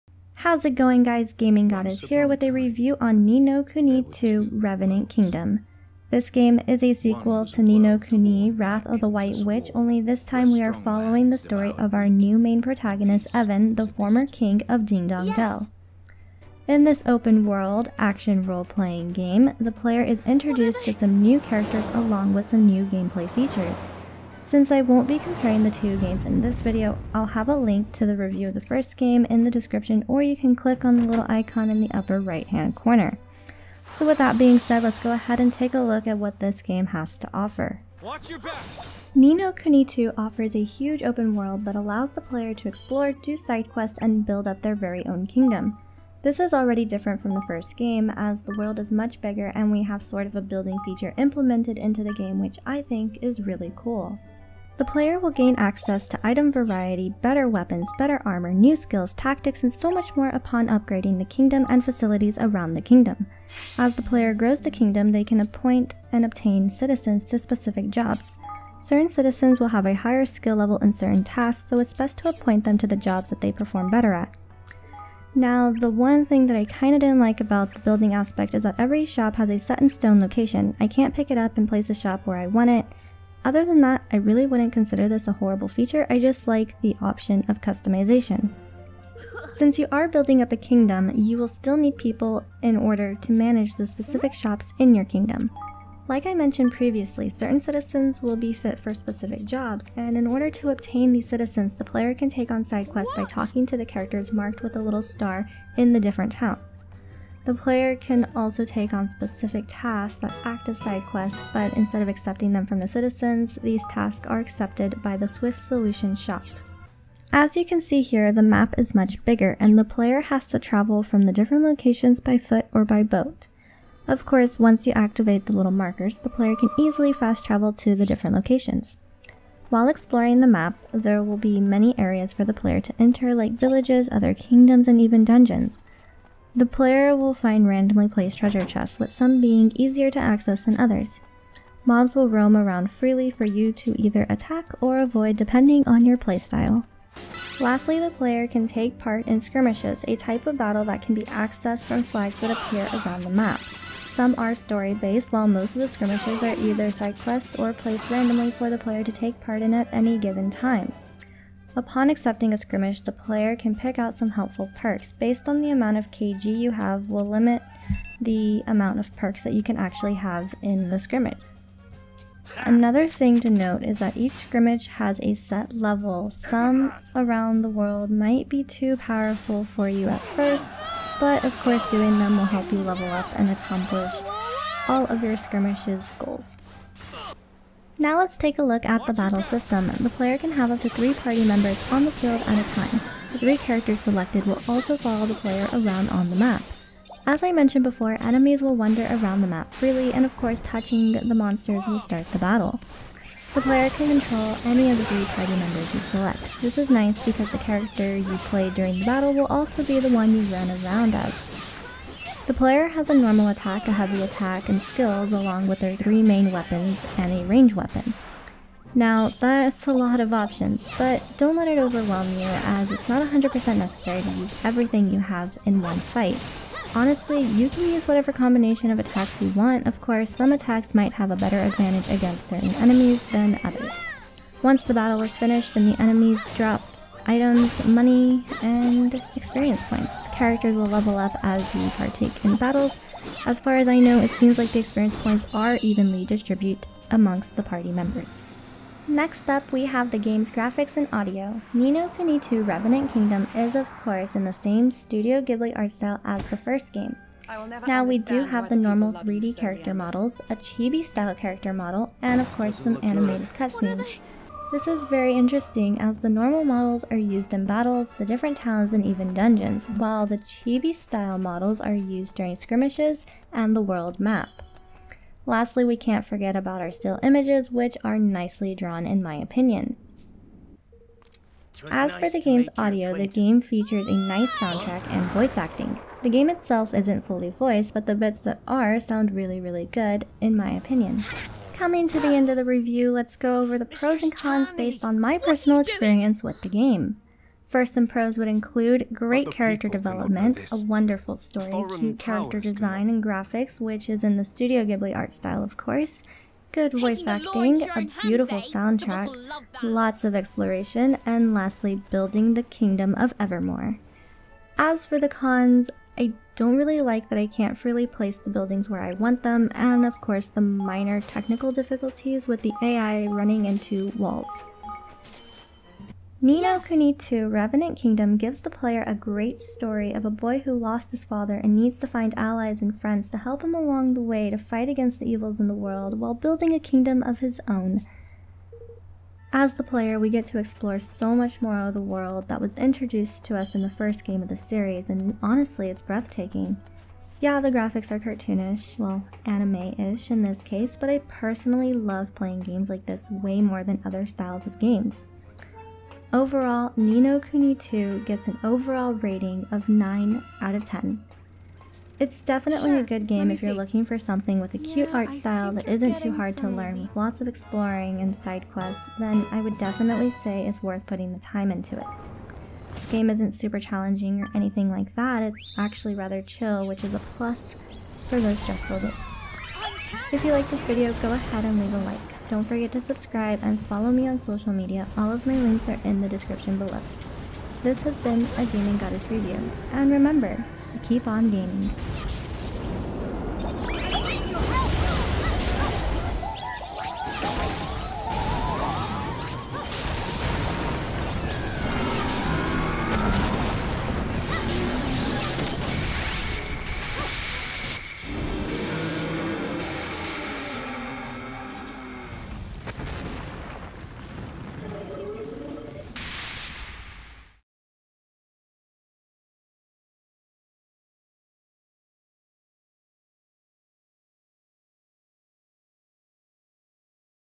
*Gaming Goddess* Ni No Kuni II Revenant Kingdom Review
Ni No Kuni II Was fun, and I hope you guys enjoy being a king as much as I did. Oh and building because I'm a builder :3